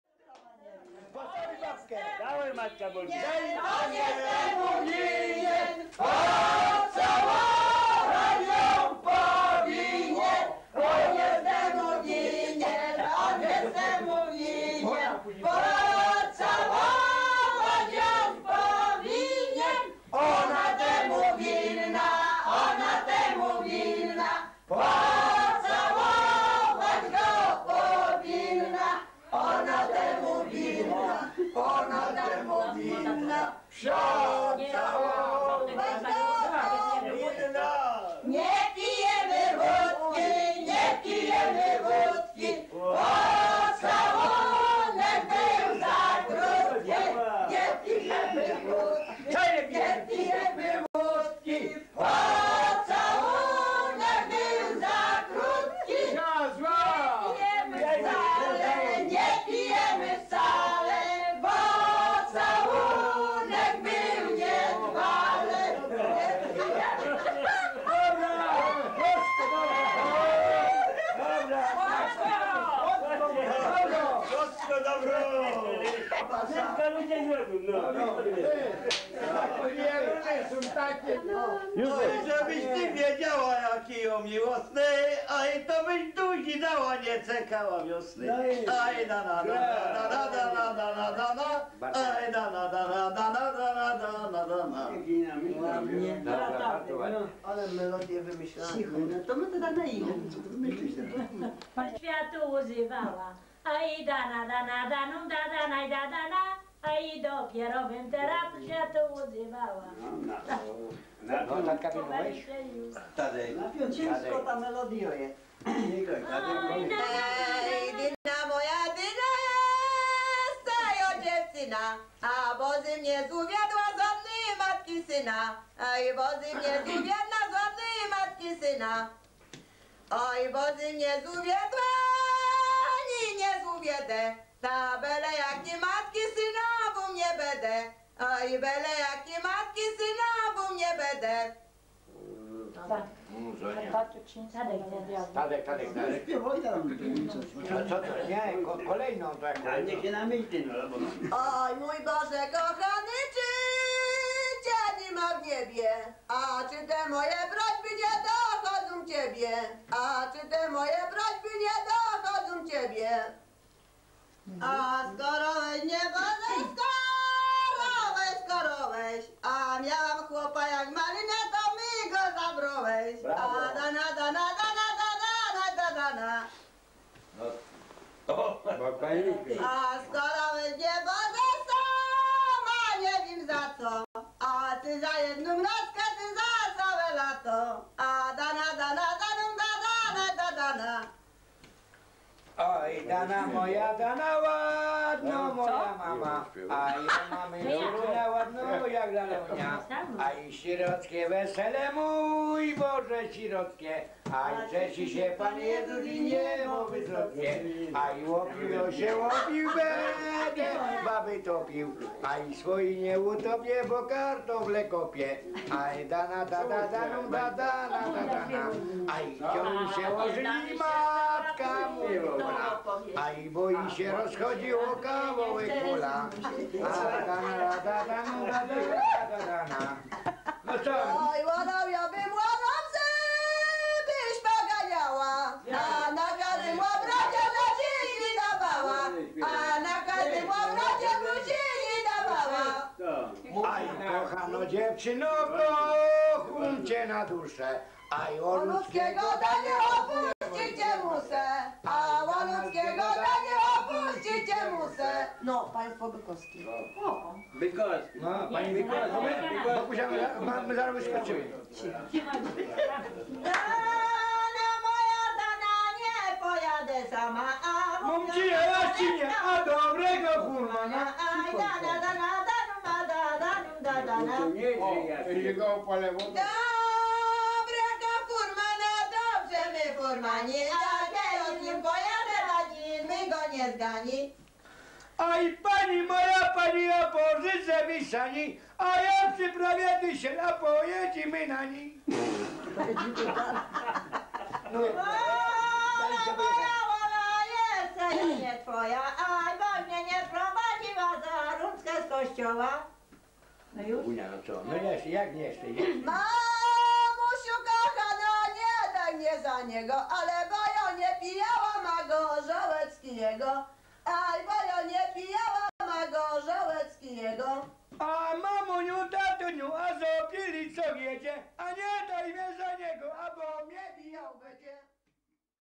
GAŁKI 1999 R. Spontaniczny koncert pięknych, weselnych PRZYŚPIEWEK ~ Ludowa biesiada pod RADOMIEM
Organizatorką biesiady była Józefa (mama BOGDY), która przewodziła Ludowemu Zespołowi z Gałek. Tak więc śpiewy i tańce u "BOGDY" mają długą tradycję. Mama i Tata Bogdy całują się na początku filmiku.